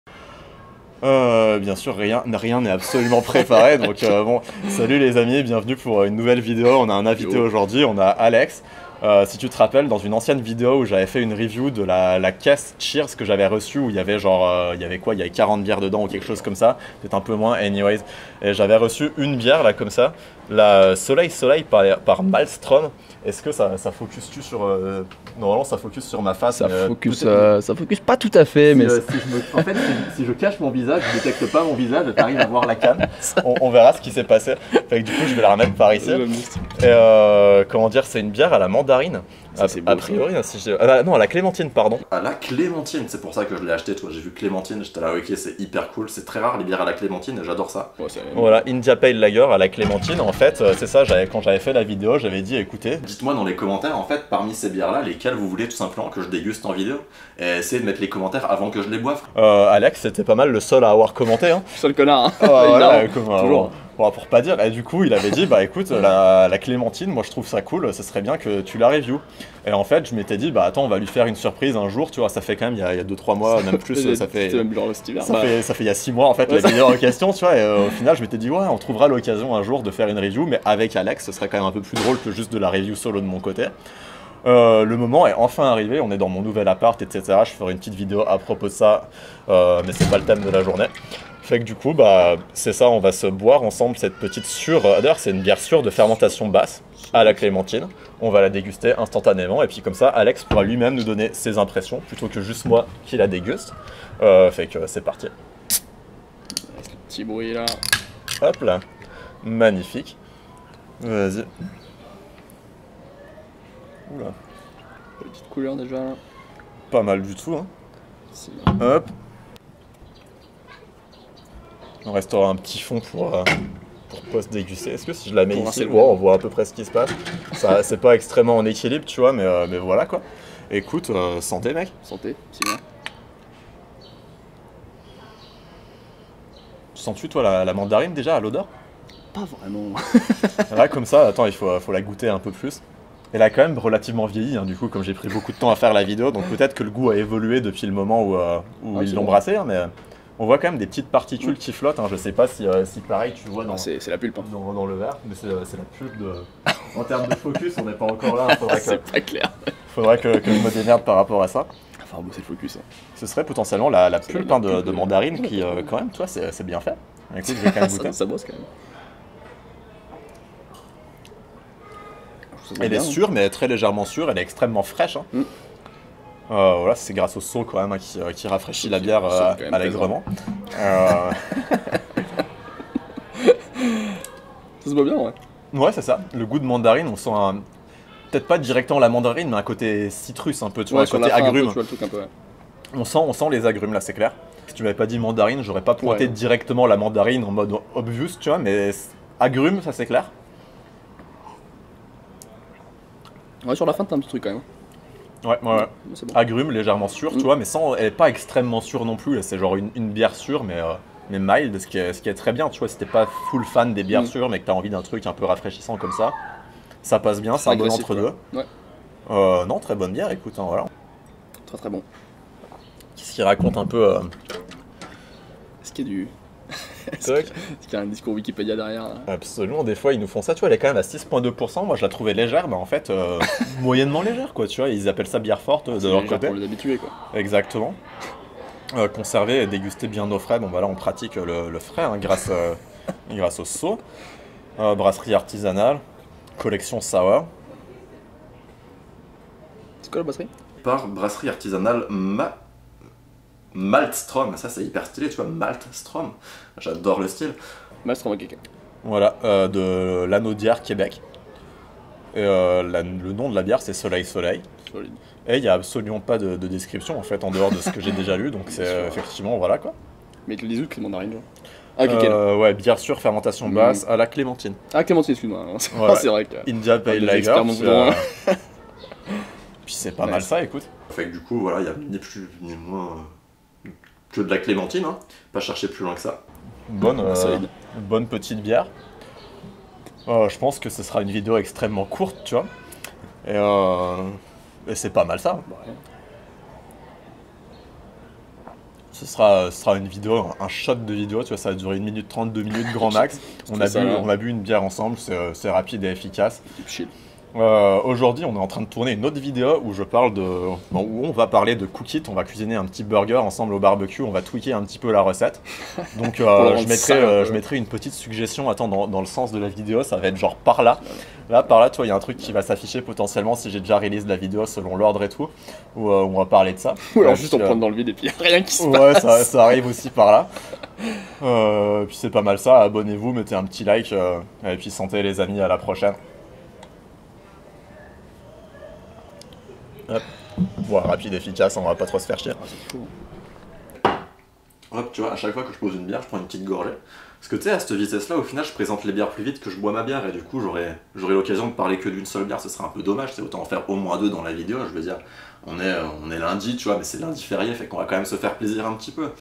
On déguste une bière SURE à la CLÉMENTINE 🍊
Brassée en exclusivité pour la Boutique Cheers, Soleil Soleil est une India Pale Lager sure à la clémentine de la Brasserie Maltstrom.

Dans une précédente vidéo je vous faisait découvrir le contenu de la caisse de bières que j'avais commendé à la Boutique Cheers au début du confinement.

Je vous avait demandé quelles bières vous vouliez que je review, et Alex avait suggéré la Soleil Soleil. C'est en sa compagnie que nous la dégustons aujourd'hui.

Zoom H2n